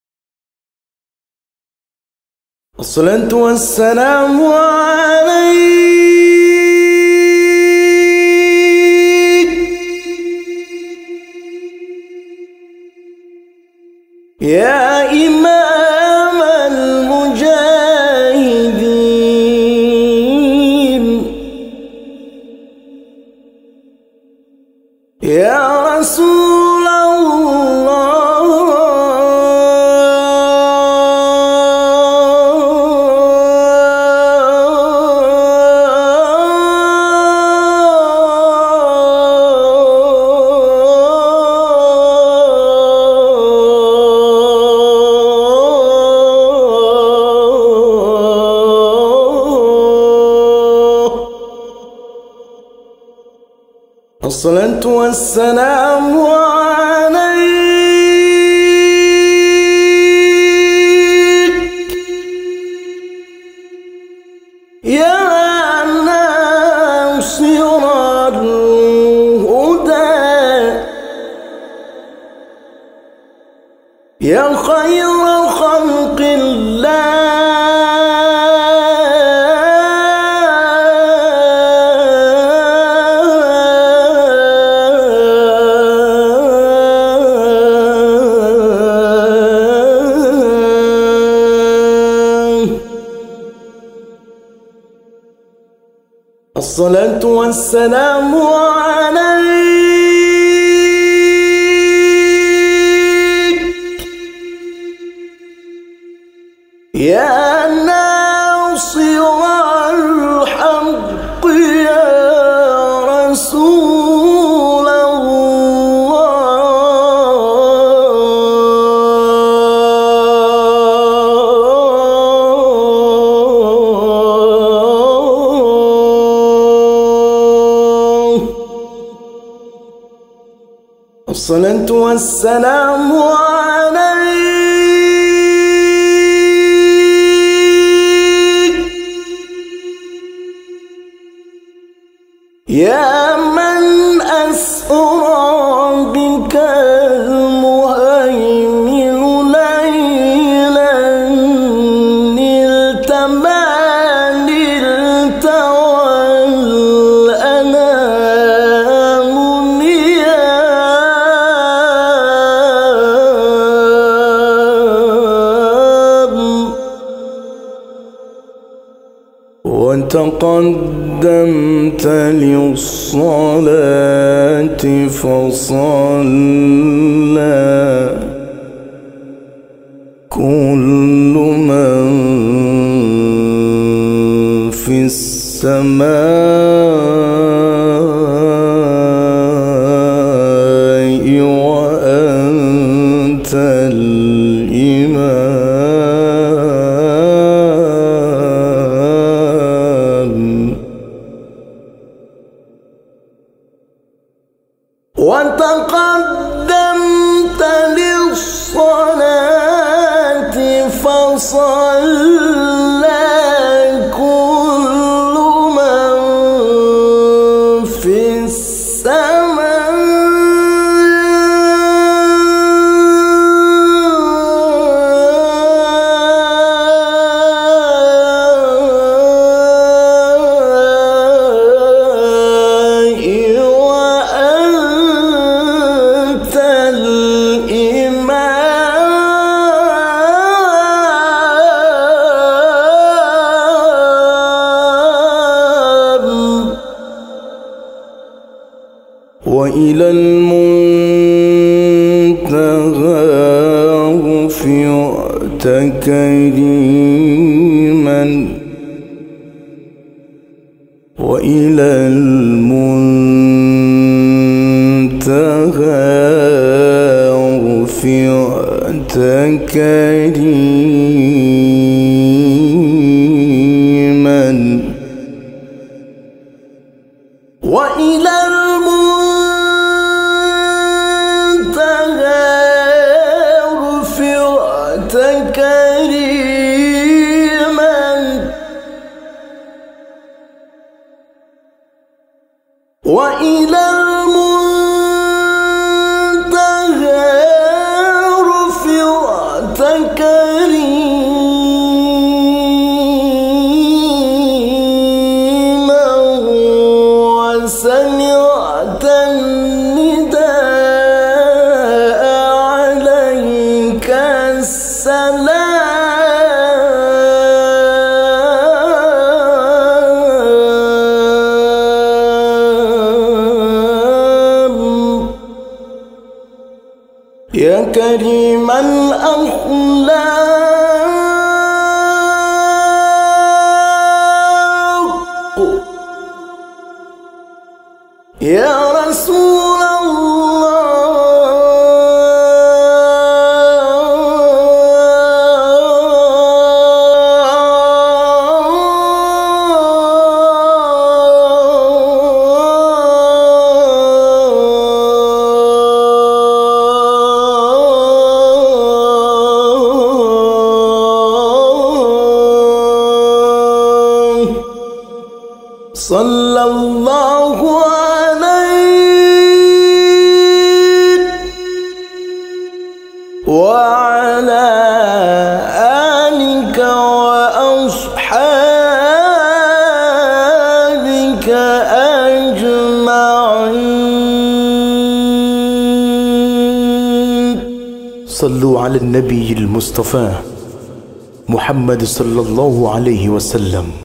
Bacaan Paling Merdu Sebelum Adzan
Bacaan tarkhim ini di kumandangkan disaat sebelum masuk waktu sholat lima waktu di sebelum Adzan. maksudnya adalah tentang segala pujian dan sanjungan kepada baginda Nabi saw.

Alunan Qur'an merdu sepanjang masa